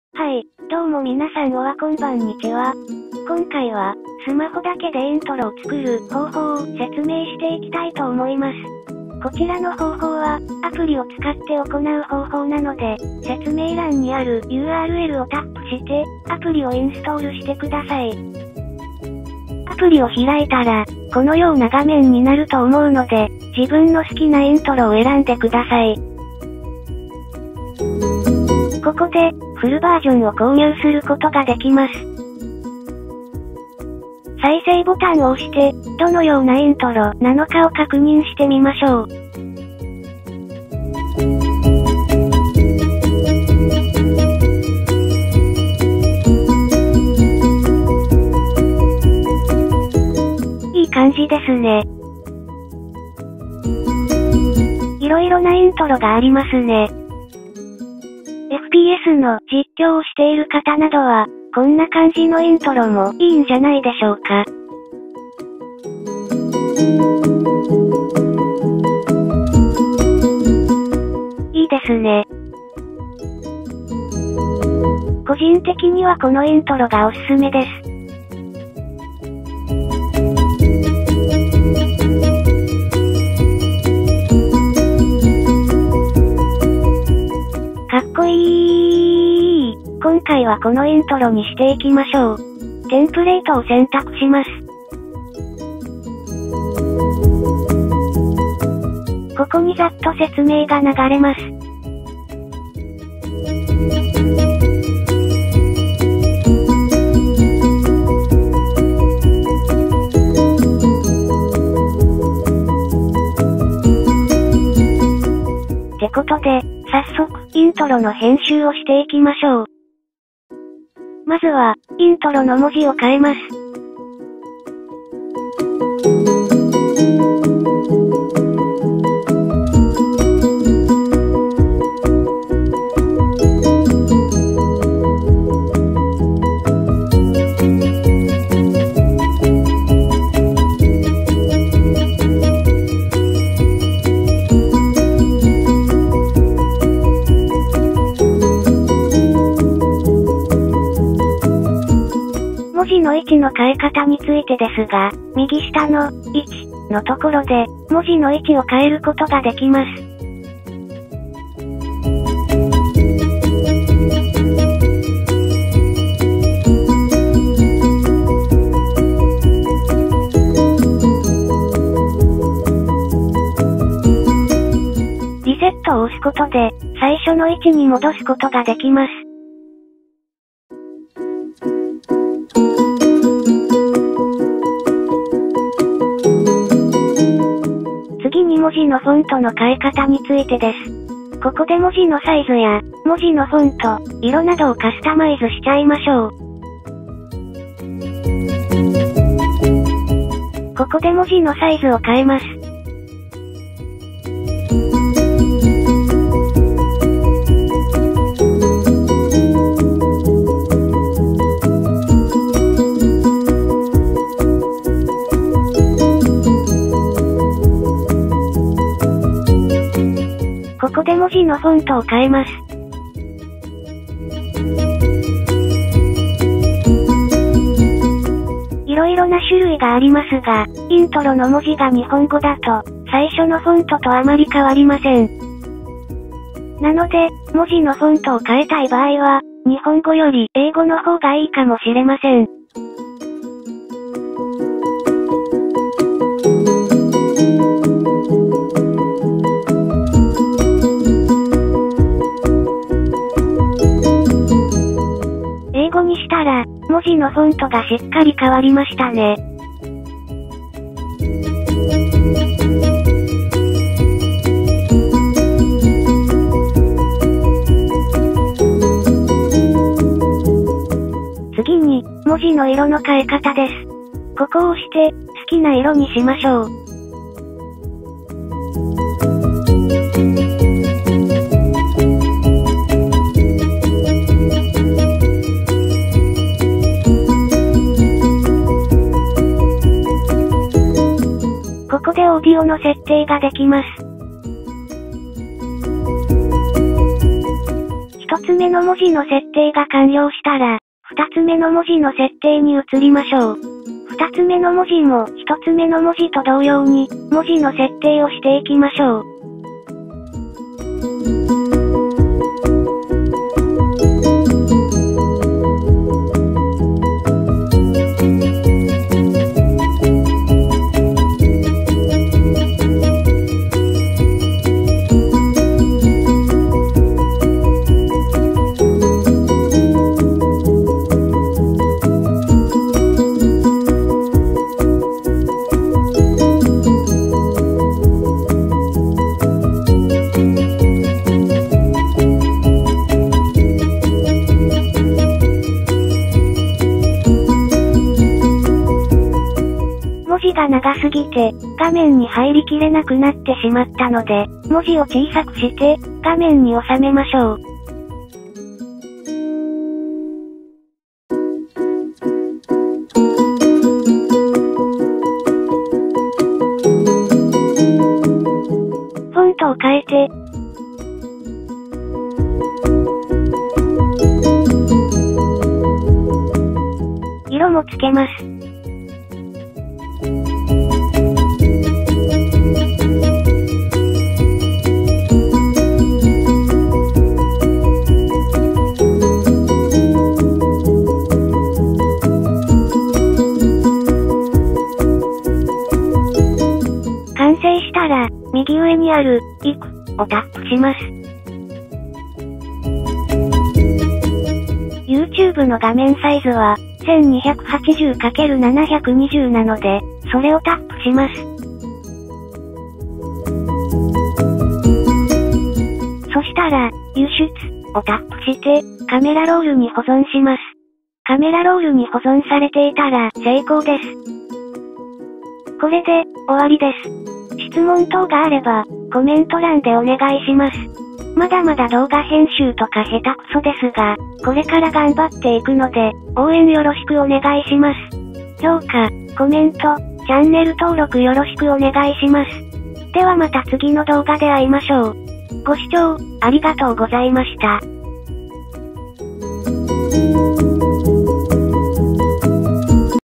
【ゆっくり解説】スマホだけでカッコいいイントロの作り方【introDesigner】
イントロデザイナーダウンロードはこちらから☞
まだまだ初心者ですが、これから動画投稿頑張ります！
質問があれば、コメント欄でお願いします。
ではまた次の動画で会いましょう！